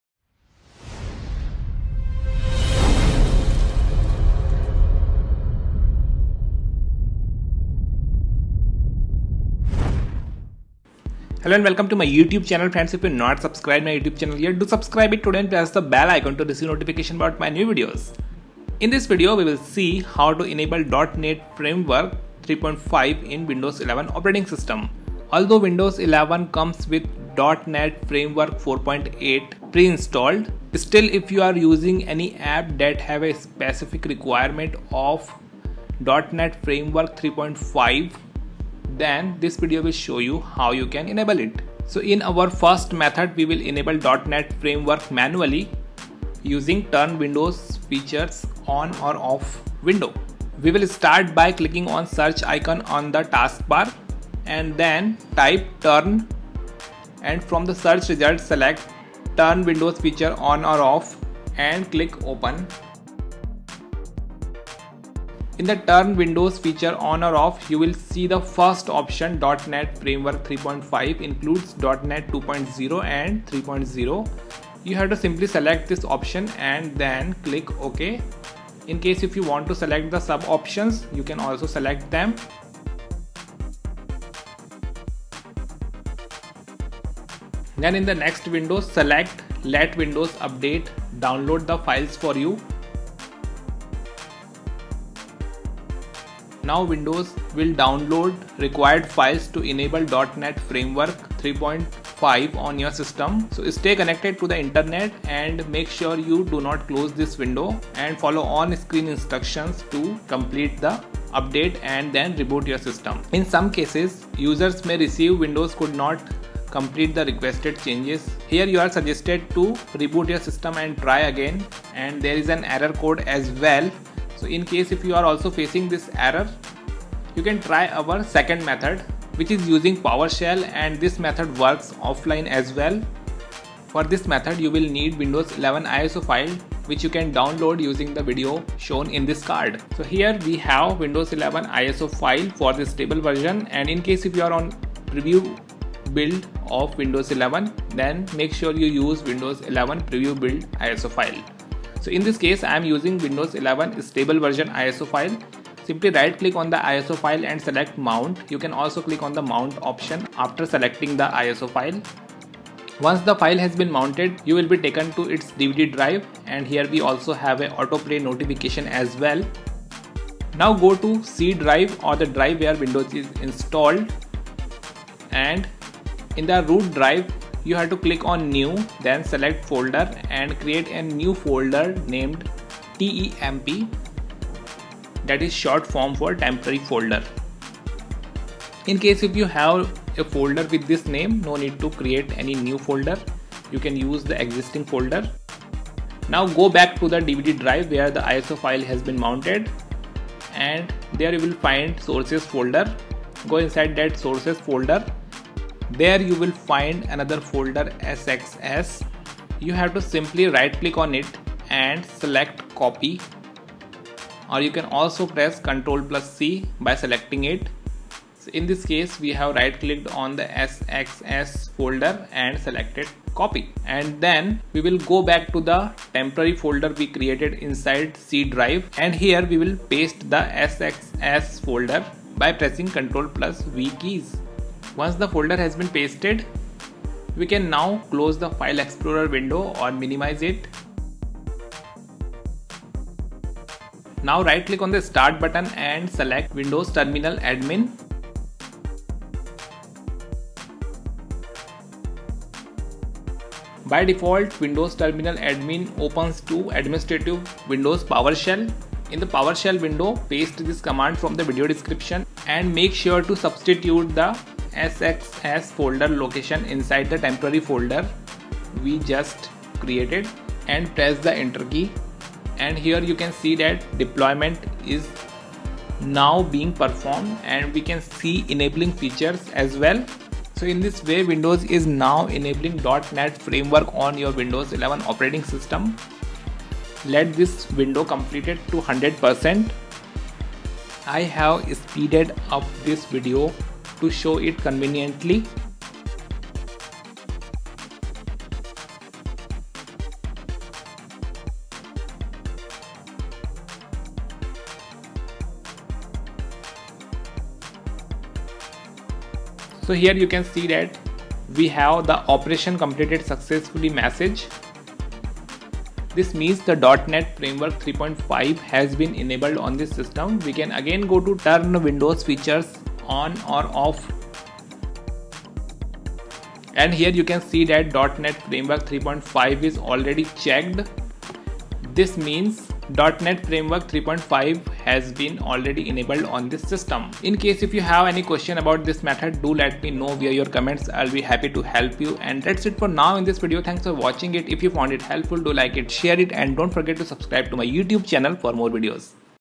Install .NET Framework 3.5 in Windows 11
This video will show you how to enable or install .NET Framework 3.5 in Windows 11.

PowerShell command to execute:

dism.exe /online /enable-feature /featurename:NetFX3 /All /Source:c:\temp\sxs /LimitAccess

In above command, replace c:\temp\sxs with the actual location of sxs folder on your system, in case if it is at different location.

Chapters:
00:00 Intro
00:41 Enable .NET Framework 3.5 manually
02:10 Enable .NET Framework 3.5 using PowerShell
05:42 End screen